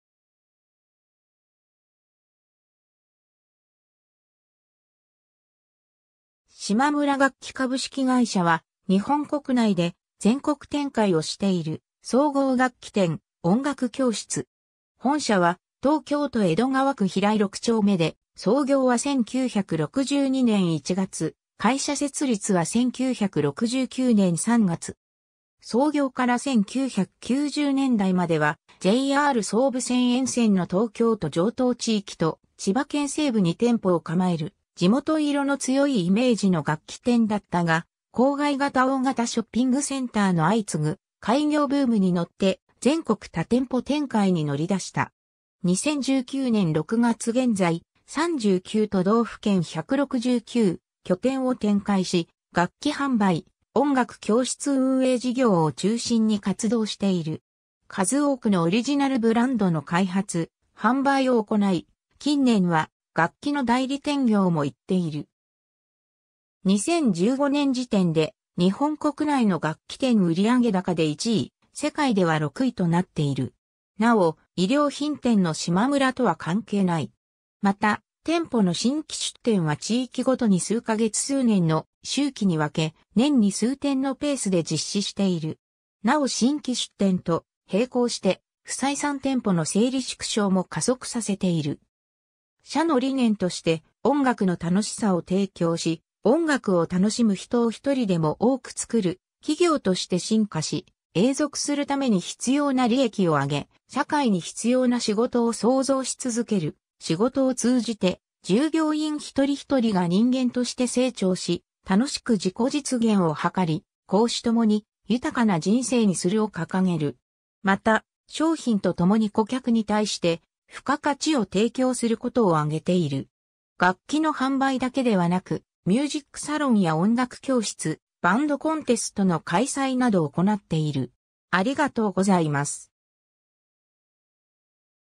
島村楽器
平井_(江戸川区)
島村楽器株式会社（しまむらがっき）は、日本国内で全国展開をしている総合楽器店・音楽教室。
本社は東京都江戸川区平井六丁目で、創業は1962年1月、会社設立は1969年3月。
 創業から1990年代までは、JR総武線沿線の東京都城東地域と千葉県西部に店舗を構える、地元色の強いイメージの楽器店だったが、郊外型大型ショッピングセンターの相次ぐ開業ブームに乗って、全国多店舗展開に乗り出した。
2019年6月現在、（青森県・岩手県・富山県・福井県・島根県・山口県・徳島県・高知県を除く）39都道府県169拠点を展開し、楽器販売・音楽教室運営事業を中心に活動している（音楽教室講師陣は全て自社で正社員として採用・育成）。
数多くのオリジナルブランドの開発・販売を行い、近年は楽器の代理店業も行っている。
 2015年時点で、日本国内の楽器店売上高で1位、世界では6位となっている。
 なお、衣料品店のしまむらとは関係ない。
また店舗の新規出店は地域ごとに数ヶ月～数年の周期に分け、年に数店のペースで実施している（近年はイオンモールとその系列店の新規開店や大規模改装に合わせて入居するケースが多い）。
なお新規出店と並行して「不採算店舗の整理縮小」も加速させている。
 社の理念として、「音楽の楽しさを提供し、音楽を楽しむ人を一人でも多く創る」「企業として進化し、永続するために必要な利益を上げ、社会に必要な仕事を創造しつづける」「仕事を通じて従業員一人ひとりが人間として成長し、楽しく自己実現を図り、公私共に豊かな人生にする」を掲げる。
また、商品と共に顧客に対して付加価値を提供することをあげている。
楽器の販売だけではなく、ミュージックサロンや音楽教室、バンドコンテスト（主にHotline、録れコン、アコパラ）の開催など行っている。